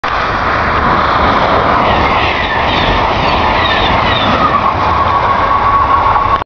NB Keystone
Heading past Jersey Avenue heading to Newark Airport and then eventually New York Penn Station with Ex-Metroliner Cab Car pulling and an AEM-7 pushing.